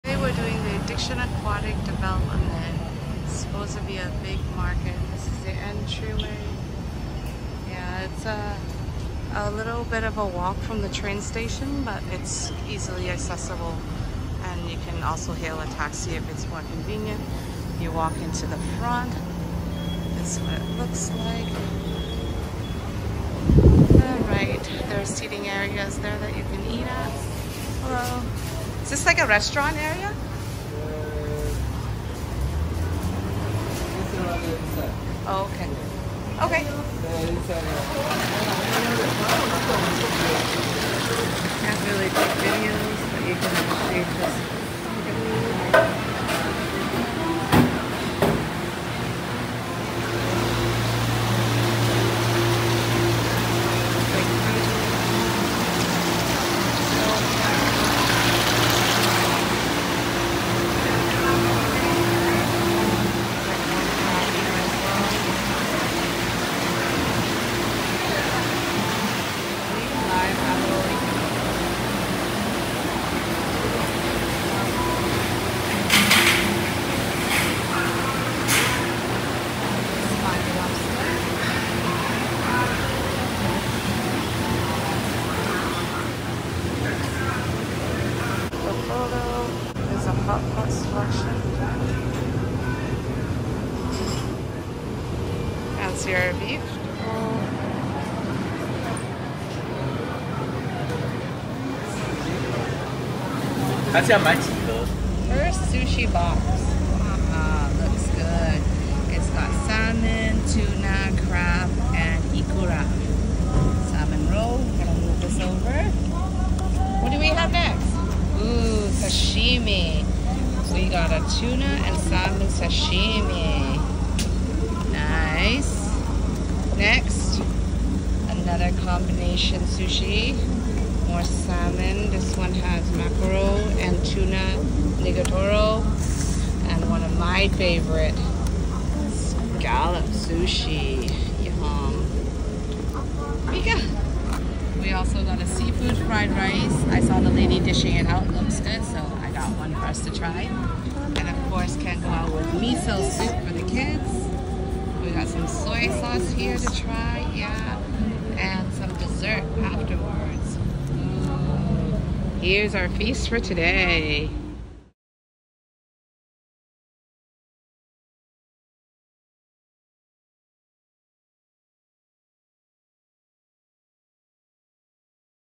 Sushi Lunch @ Addiction Aquatic Development, Taipei, Taiwan
We watched many YouTube videos and read many reviews about the Addiction Aquatic Development in Taipei, so we decided to pay a visit today. We walked around the market. The market was nice, organized, and pretty clean; however, it was a little bit confusing in the entryway. Look for the entrance signage before trying to enter the market. It has only one way in and one way out. The ready-made foods area is the busiest and is highly congested as it’s closed to the checkout area. Also, video recording is not permitted inside the market, so turn off your video.

We walked through the market and picked out a bunch of sushi and sashimi to try. The sushi and sashimi tasted alright. Nothing special. But the freshness was missing too. I am not sure how long the fish had been sitting, and the preparation may not have been handled with the utmost care. I’ve had better sushi elsewhere.

At the market, seating was limited. Most stand-up tables. The only available seats were in front of the development, but most of the seats were actually in the sun, so if you come there on a hot day, it will be very unpleasant.

Technically speaking, the development is just an upscale, high-end grocery store. I don’t recommend going there because the price was high for average food. Also, I didn’t find the seafood very fresh there.